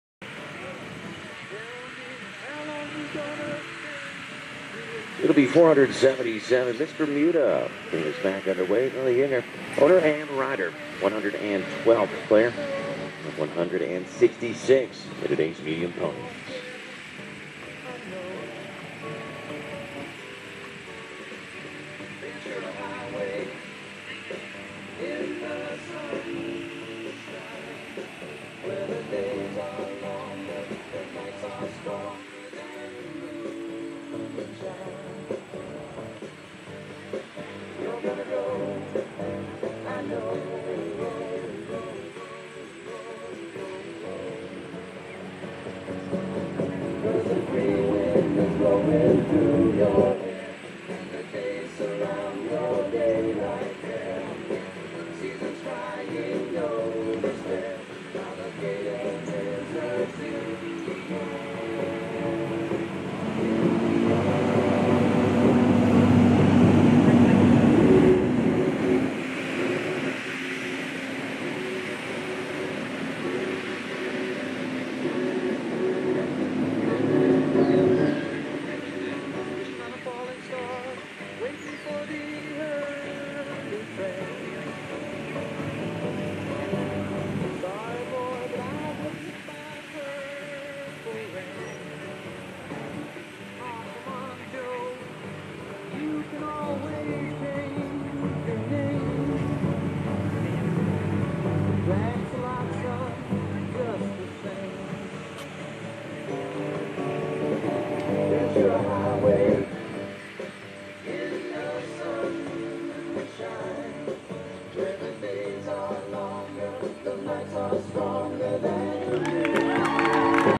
Video of MISS BERMUDA ridden by LILLY YINGER from ShowNet!
MISS BERMUDA ridden by LILLY YINGER at USEF PONY FINALS 2018.  This round is from class 20, MEDIUM PONY O/F and finished 12 of 166.